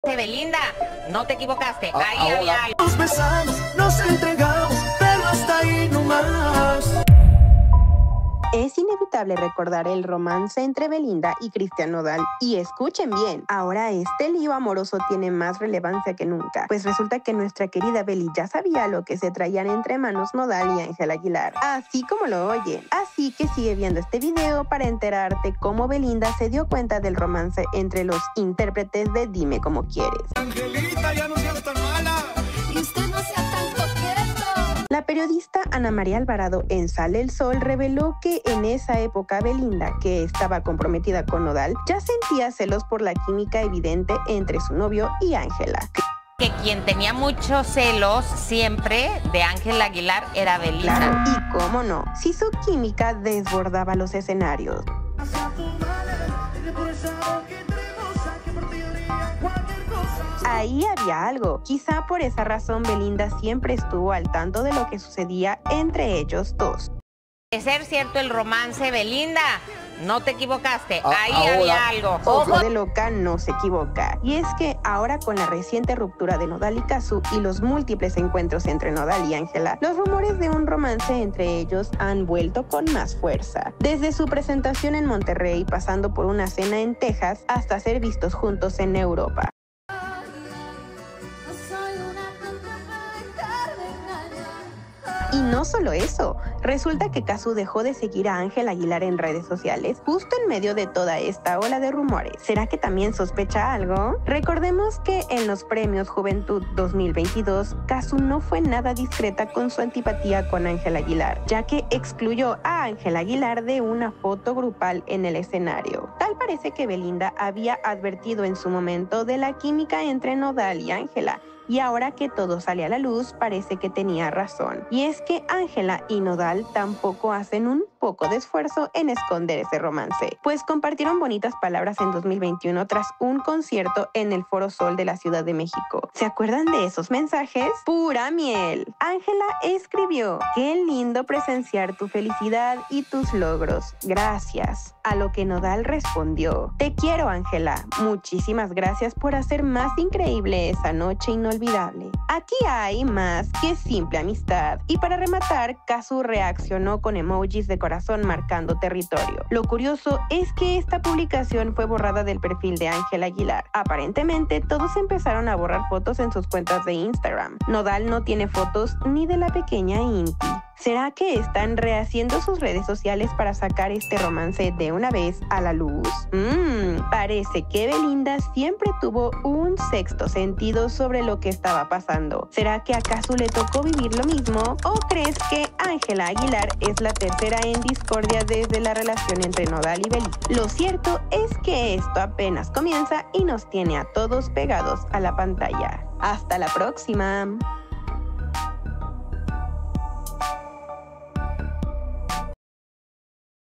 Así fue como BELINDA se dio CUENTA del romance de NODAL Y ÁNGELA AGUILAR
OJO DE LOCA NO SE EQUIVOCA